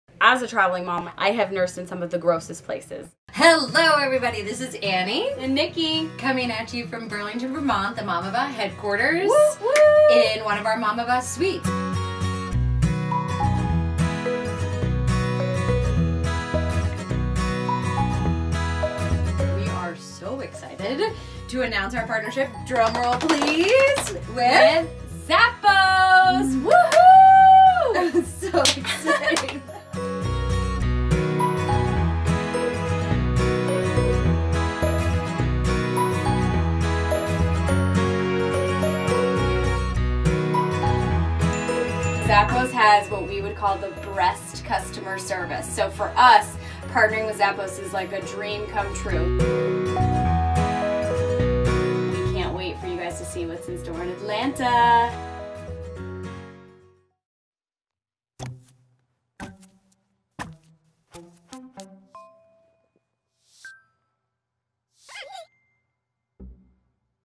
The Breast Place to Pump in ATL with Mamava & Zappos
Pumpin’ ain’t easy, especially while traveling or on the move.  Zappos and Mamava know this all too well so they have teamed up to make breastfeeding just a little bit brighter and provide a moment of solitude.

On February 11 Zappos will extended it’s award-winning customer service to jetsetting mothers by installing six lux Mamava nursing pods in the Atlanta International airport.

Nursing moms definitely deserve some extra TLC, and there's something for every booby in these Zappos pods with:

Lactation pad dispensers (to prevent any SpilledMilk)
Cleaning wipes and trash cans (to help clean up any SpilledMilk)
Custom soundtracks to help get the milk flowing (think ocean waves, baby giggles, etc)
Happiness quotes to cheer mamas up (in case of any SpilledMilk)
New engaging Pumpcasts to keep mamas entertained through the pump